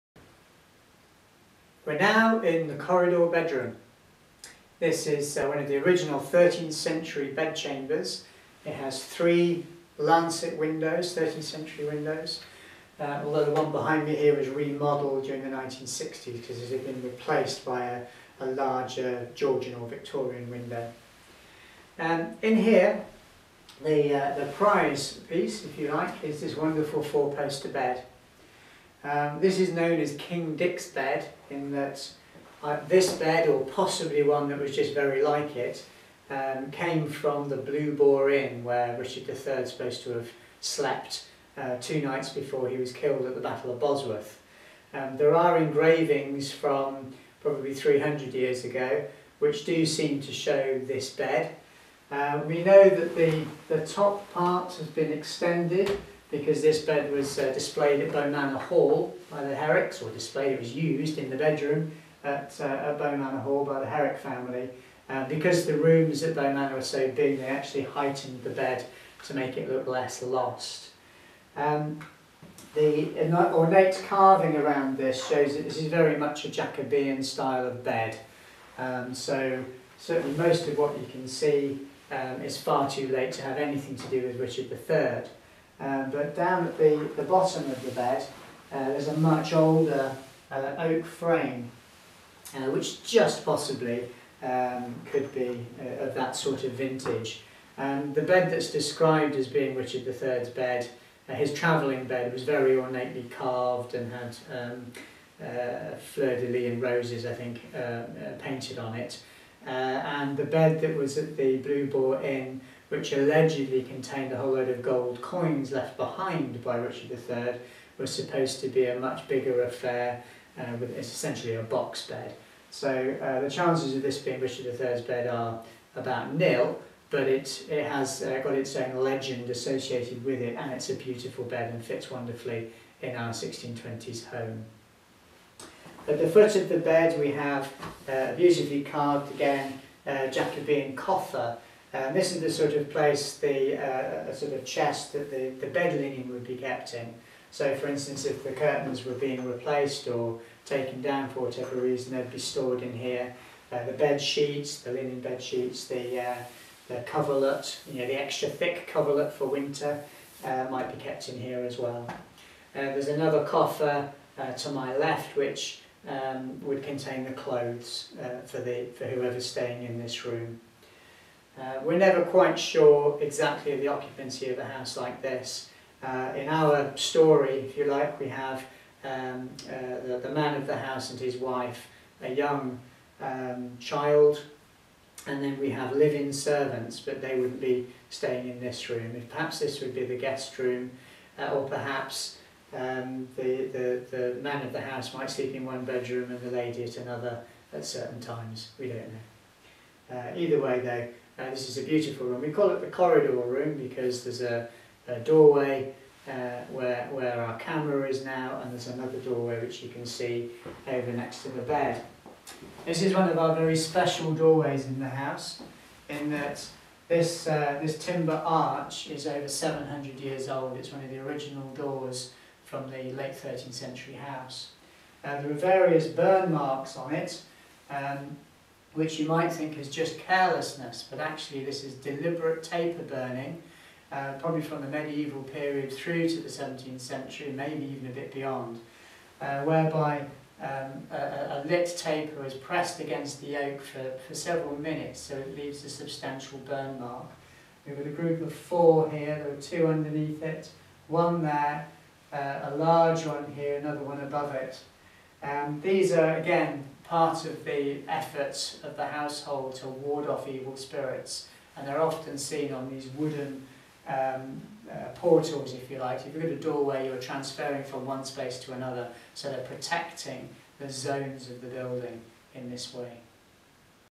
The Corridor Bedroom and King Dick's Bed at the 1620s House
This 13th century bedchamber is one of the oldest rooms in the house. The arched doorway is part of the original 13th century structure and features several taper marks, made to drive away evil spirits.
It contains the star piece of the house, known as King Dick's Bed. Legend has it that King Richard III slept in the bed at the Blue Boar in before the Battle of Bosworth.